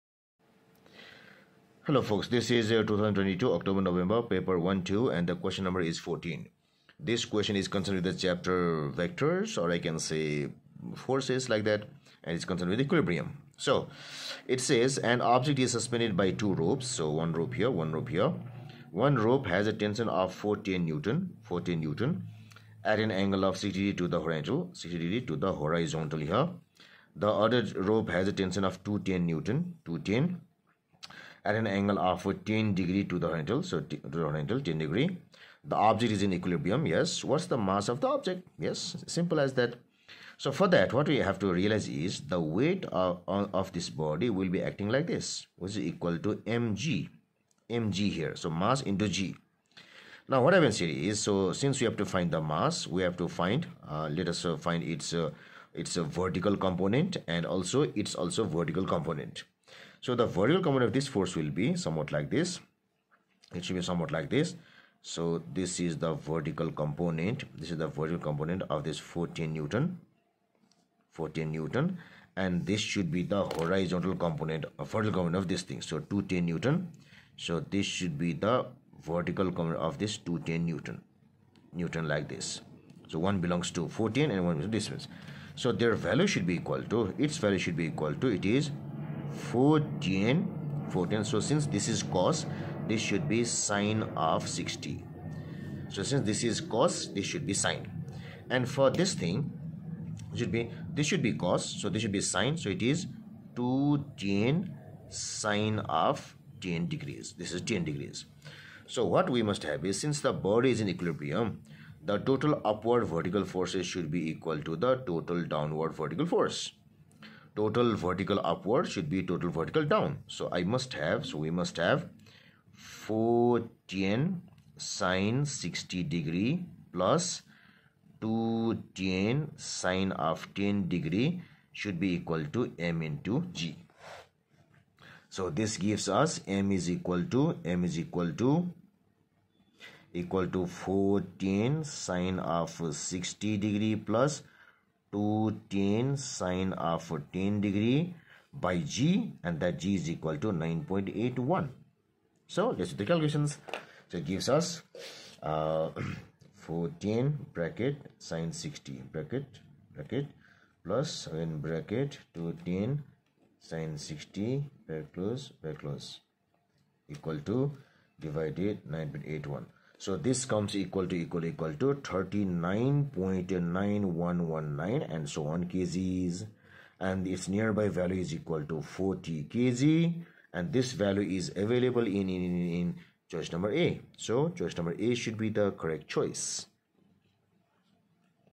2022 CAIE AS & A level October November Physics Paper 12 Q N 14 (9702/12/O/N/22) by Sajit C Shakya
Solution to 2022 CAIE AS & A level October November Physics Paper 12 Q N 14 (9702/12/O/N/22) by Sajit Chandra Shakya from Nepal

Chapter - VECTORS and FORCES

An object is suspended by two ropes. One rope has a tension of 410 N at an angle of 60 to the horizontal. The other rope has a tension of 210 N at an angle of 10 to the horizontal.

The object is in equilibrium.

What is the mass of the object?

A 40 kg B 42 kg C 390 kg D 410 kg